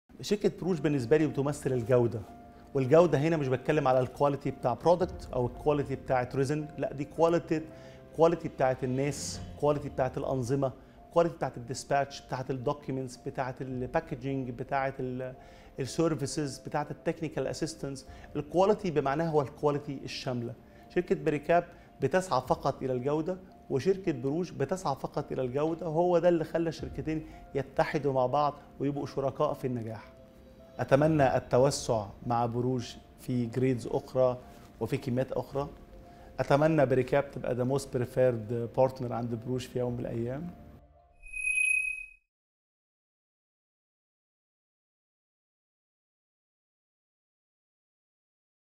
Customer Testimonial - Bericap
عميلنا لسنوات طويلة، يريكاب، يتجاوز توقعات الزبائن باستمرار بفضل مرونة حلول بروج. والأهمّ من ذلك أنّ "بيريكاب" و"بروج" يتشاركان القيم نفسها، ما يضمن التسليم الفعّال للمنتجات النهائية.

Bericap, our long-term customer, continuously exceed customers’ expectations, thanks to the agility of Borouge solutions. Most importantly, Bericap and Borouge have shared values which ensure efficient delivery of finished products.